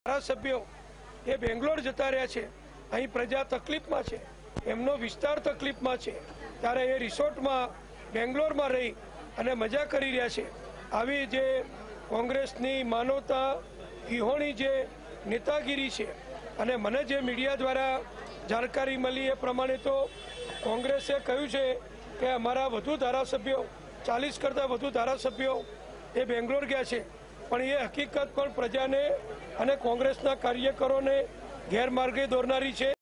Deputy CM Nitin Patel growls on Congress minister's Bengaluru visit | Vtv News
Deputy CM of Gujarat Nitin Patel growls on Congress minister's Bengaluru visit as the party in Gujarat has desolated and many ministers of Gujarat Congress are resigning and moving to Gujarat BJP.